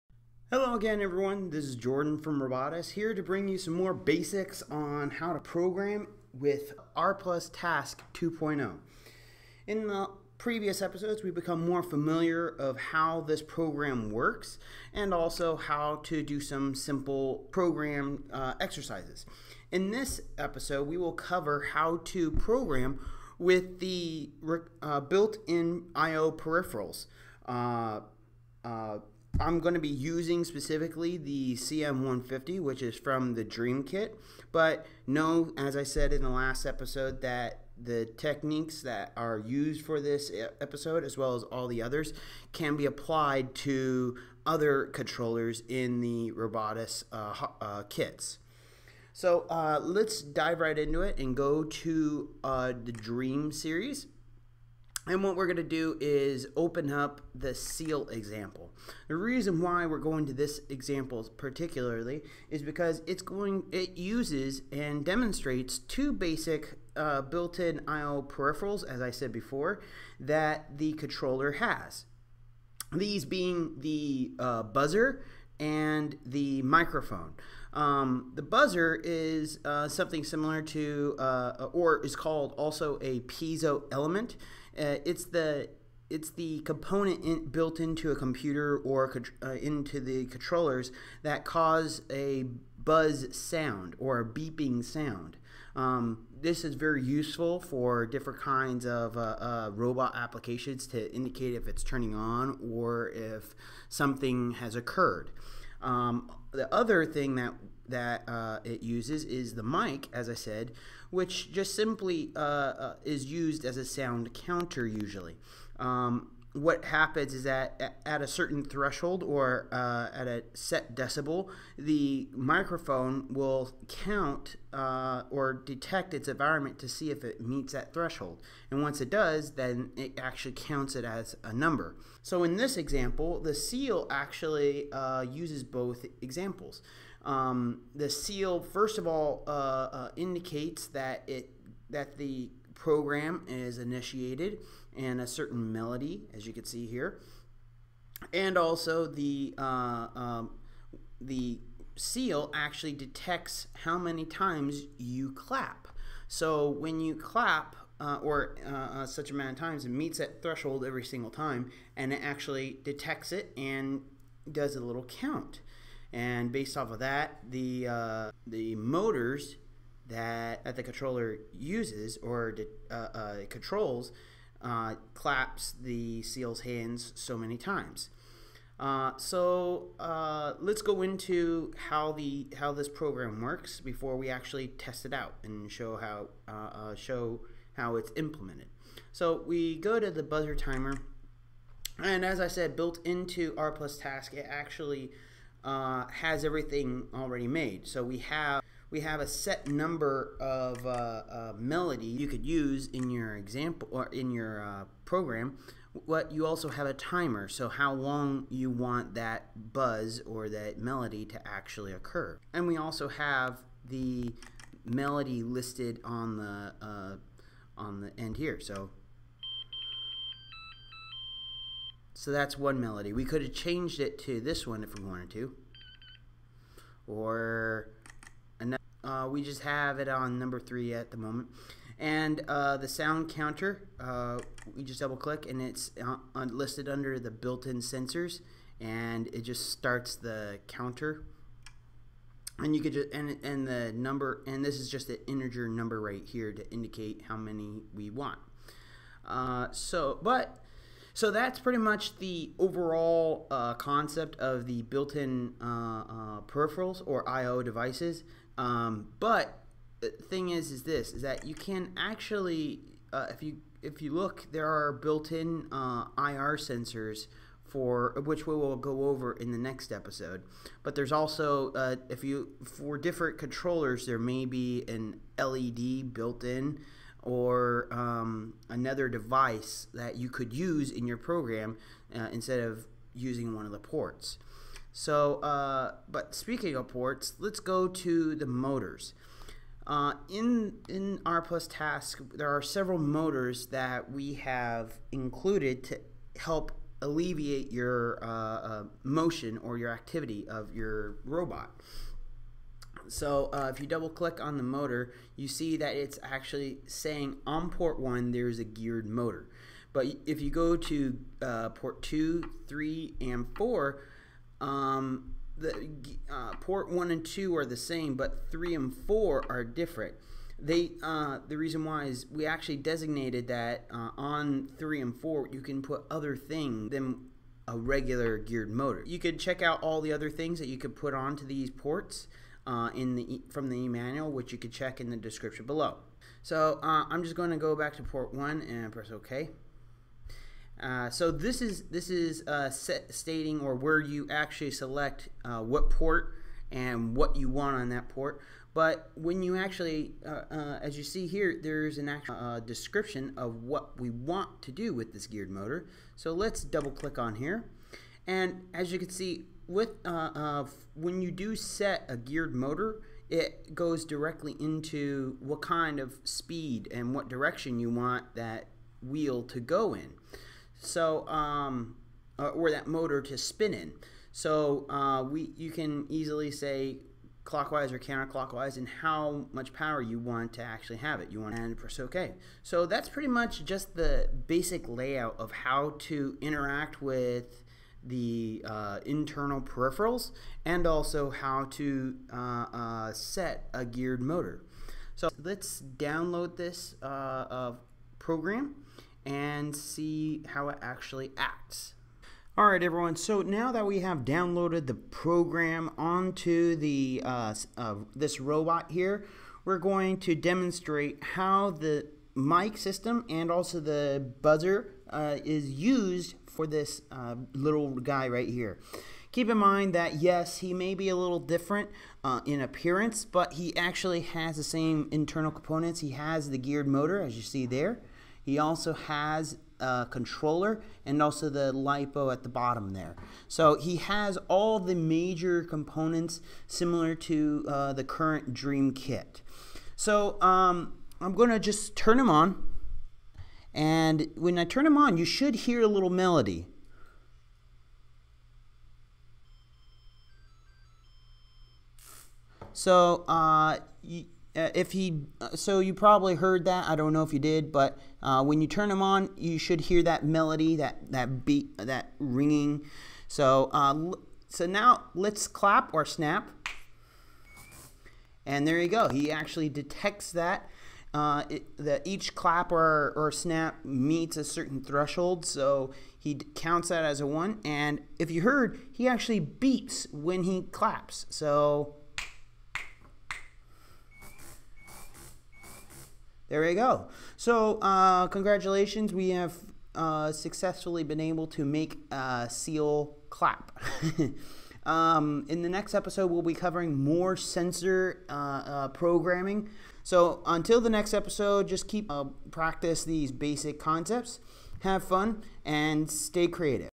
R+ TASK 2.0 Tutorial: I/O Peripherals
This R+ TASK 2.0 Tutorial goes over how to use CM series controllers built in I/O Peripherals in your task programs.